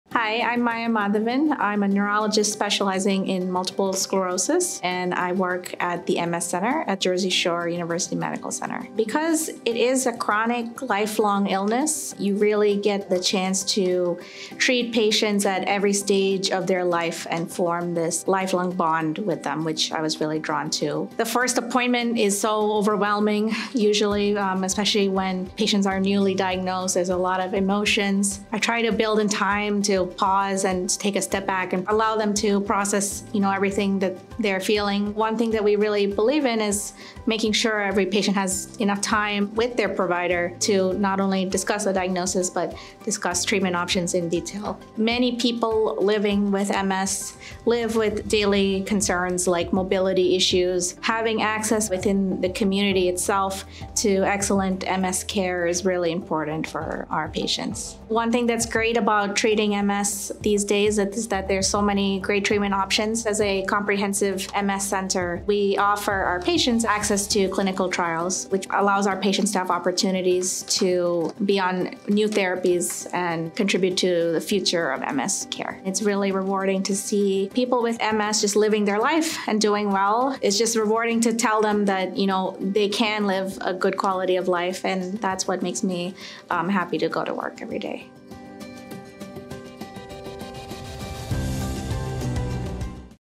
Maya Madhavan, M.D. - About the Provider
Dr. Maya Madhavan is a neurologist specializing in multiple sclerosis and neuroimmunology at the Hackensack Meridian Neuroscience Institute at  Jersey Shore University Medical Center. She introduces herself, her speciality, and her philosophy of care.